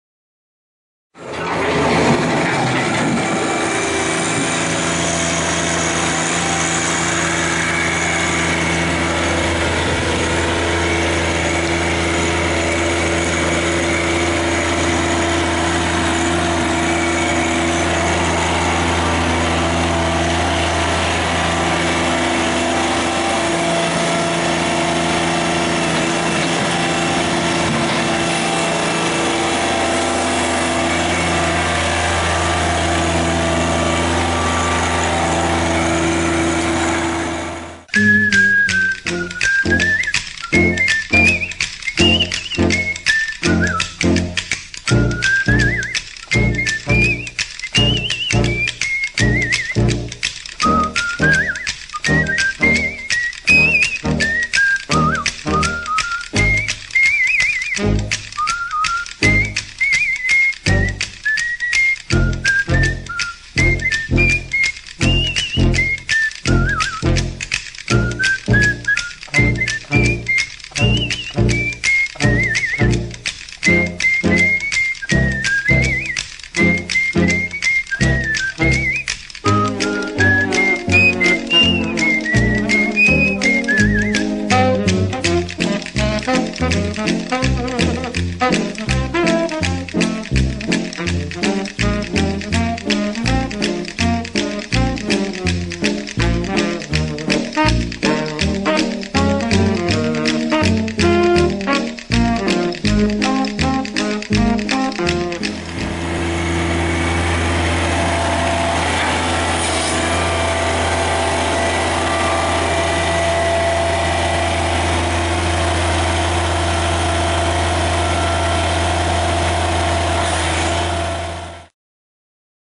Wood Mover!
Moving 4 cords and saving me hours of back pain!